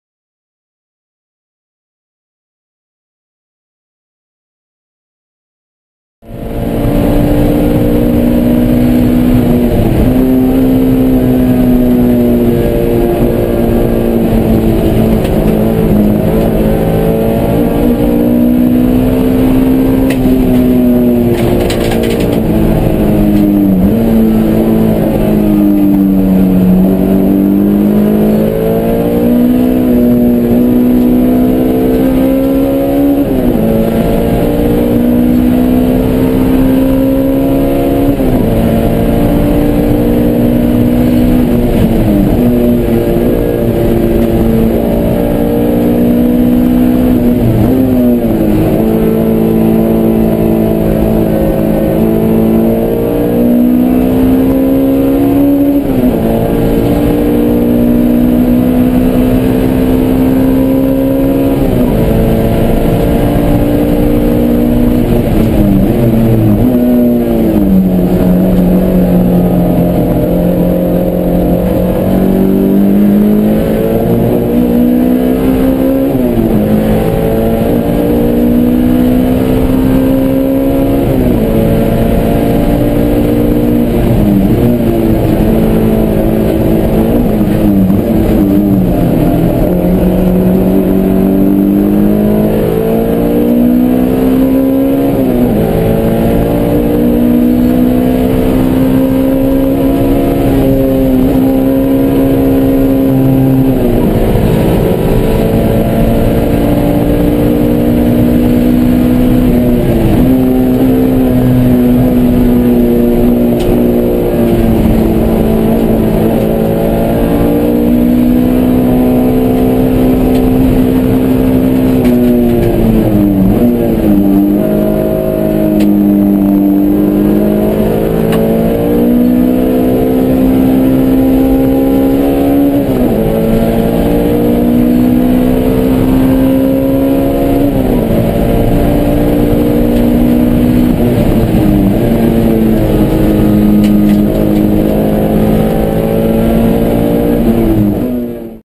Wynn Suebhongsang - Barber Motorsports - 1:39.664 - NASA TTC Honda S2000 - Oct 2016
I won the NASA Mid South 2016 Championship in TTC at this event. Huge thanks to my partners that make this all possible - SujaOne Motoring / Deft Motion / Papaya Motorsports / Grams Performance & Design / Menchie's Frozen Yogurt.